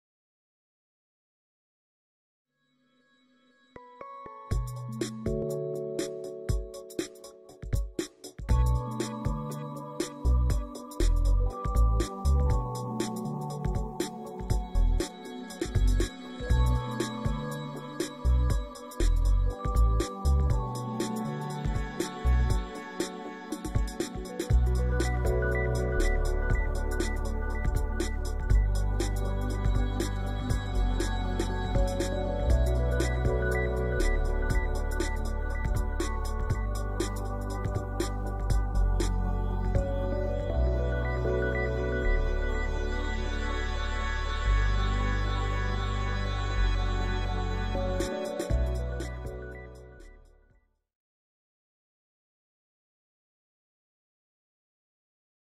Keyboard and Mouse Tray T Bird Q03
A versatile, knob-adjusted arm provides flexible comfort in the T-Bird Q03 keyboard and mouse tray. The durable surface space is large enough to accommodate a keyboard and mouse, with a see-through liftable mouse pad so you can add notes or photos beneath! The entire articulating unit features 360 degree rotation for maximum adaptability, and a padded wrist guard for ergonomic comfort. Mounting hardware is included; electronic accessories are not.